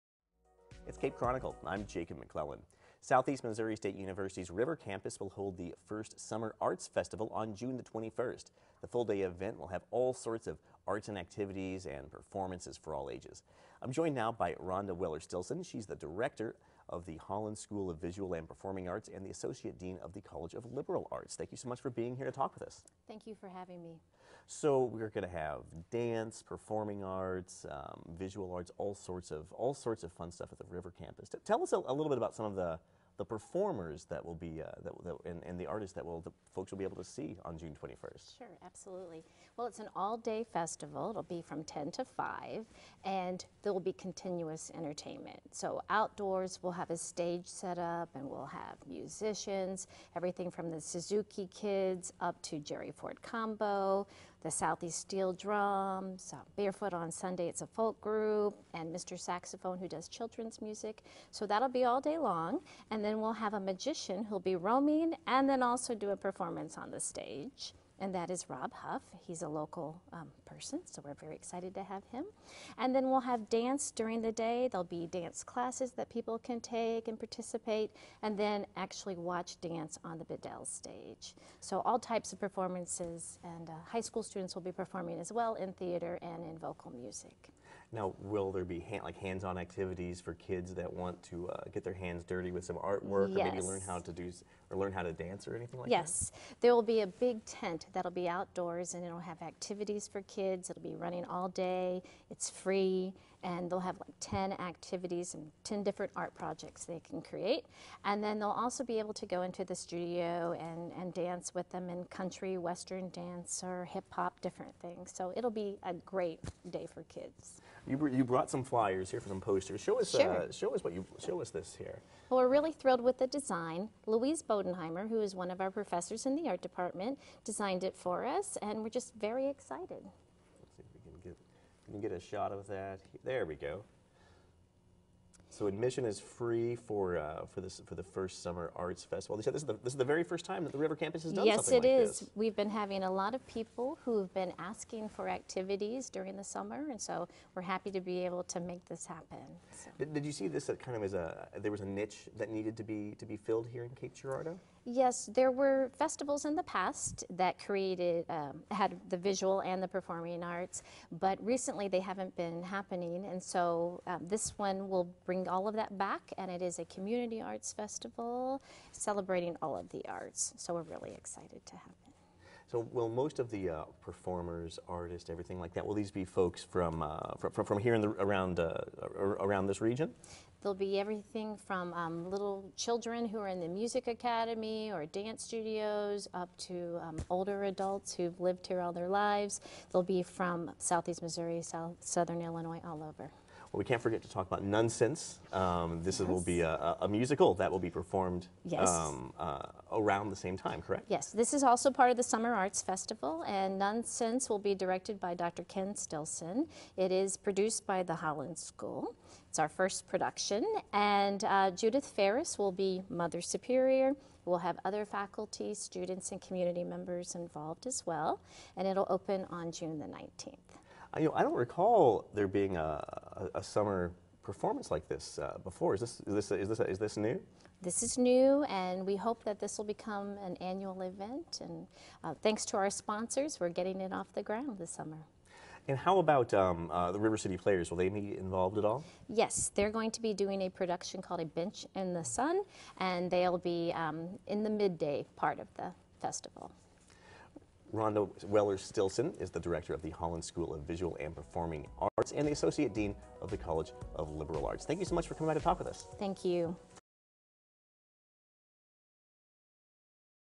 Summer Arts Festival, River Campus, Rhonda Weller-Stilson - Cape Chronicle
Southeast Missouri State University's River Campus is launching its first ever full-day Community Summer Arts Festival June 21 that will feature live music, dance, theatrical performances, magic, visual arts displays, hands-on family-friendly art expression activities and much more. This community festival is free and open to all ages.